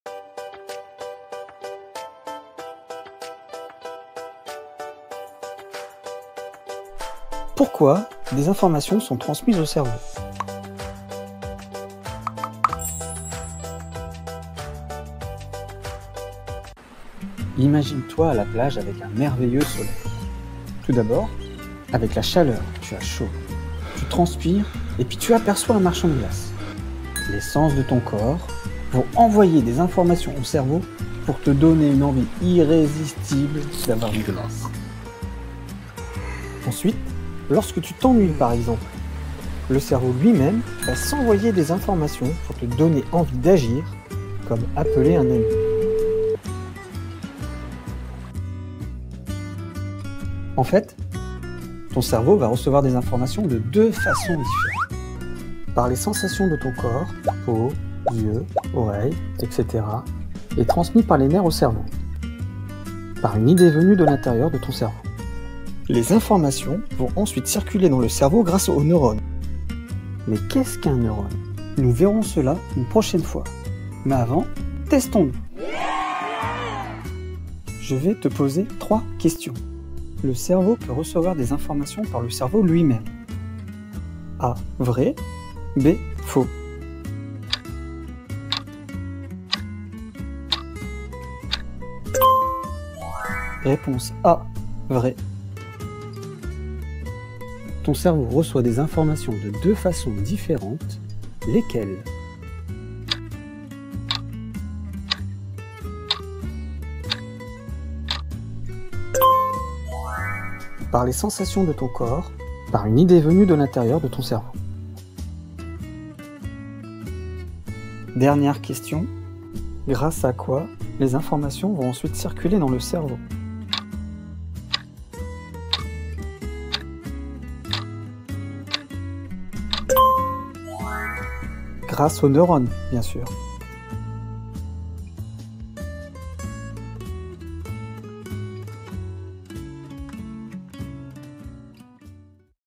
4 - Pourquoi des informations sont transmises dans le cerveau ?
Comprendre comment des informations arrivent dans le cerveau !

Des informations sont transmises au cerveau par les sens du corps et par une idée venant de l'intérieur de ton cerveau.